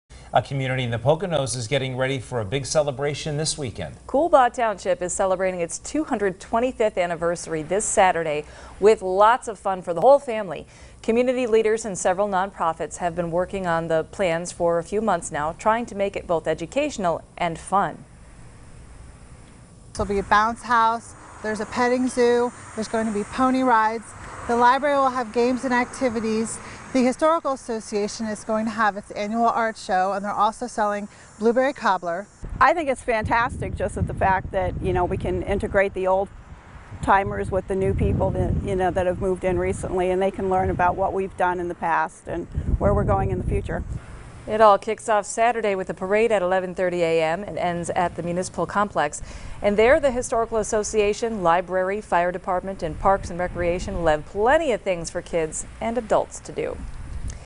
Coolbaugh Township Anniversary Celebration
COOLBAUGH TOWNSHIP, MONROE COUNTY (WBRE/WYOU-TV) A community in the Poconos is getting ready for a big celebration this weekend.

Coolbaugh Township is celebrating its 225th anniversary this Saturday with lots of fun for the whole family.  Community leaders and several non-profits have been working on the plans for a few months trying to make it both educational and fun.   It all kicks off with a parade at 11:30am that ends at the municipal complex.  There the historical association, library, fire department and parks and recreation will have plenty of things for kids and adults to do.

“They'll be a bounce house, petting zoo, pony rides.  The library will have games and activities.  The historical association is going to have its annual art show and they're also selling blueberry cobbler,” explains Ann Shincovich, Director Pocono Mountain Public Library
“I think it's fantastic just in the fact that we can integrate the old-timers with the new people that have moved in recently and they can learn about what we've done in the past and where we're going in the future,” noted Clare Colgan, Coolbaugh Township Supervisor

The event is free and  runs until 4pm.  There will be shuttles running from the nearby firehouse to help with parking.